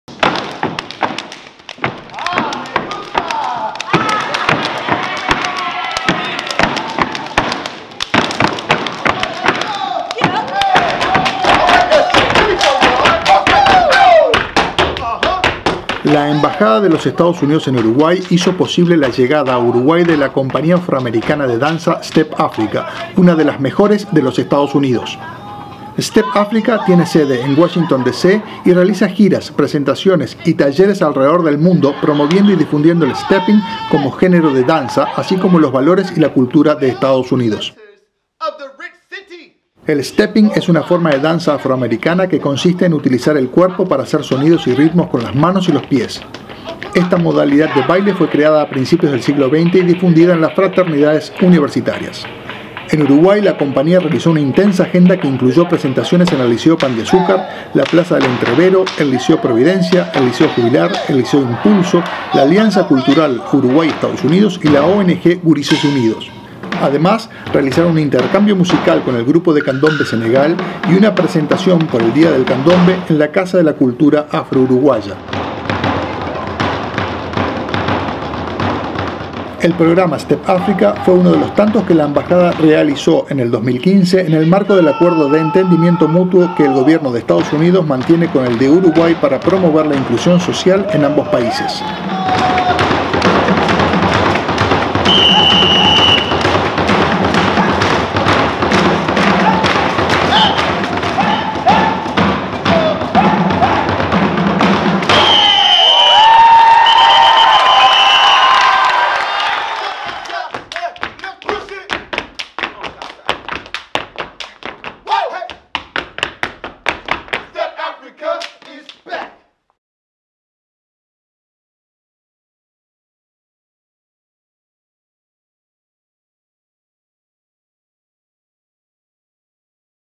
Step Afrika! en Uruguay
La Embajada de los Estados Unidos en Uruguay hizo posible la llegada de la compañía afroamericana de danza Step Afrika!, una de las mejores de los Estados Unidos con sede en Washington DC.